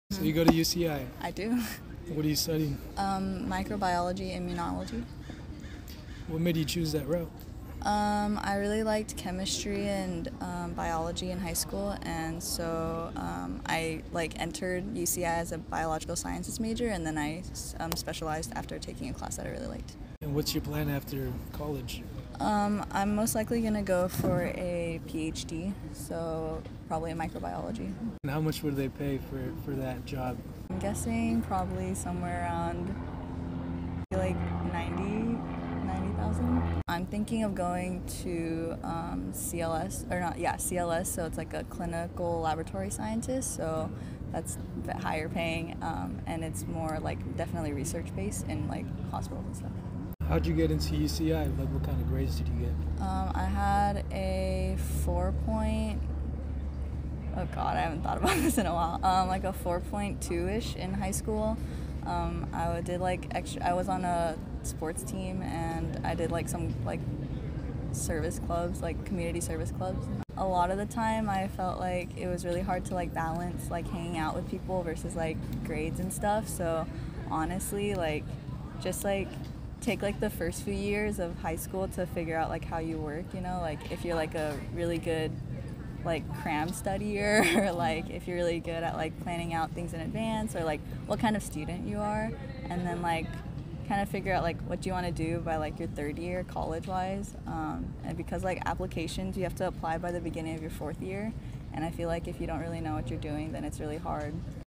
Microbiology Major Will Make $90,000 After Graduating!
Had a chat with a microbiology major. She gave great advice on how to be a better student.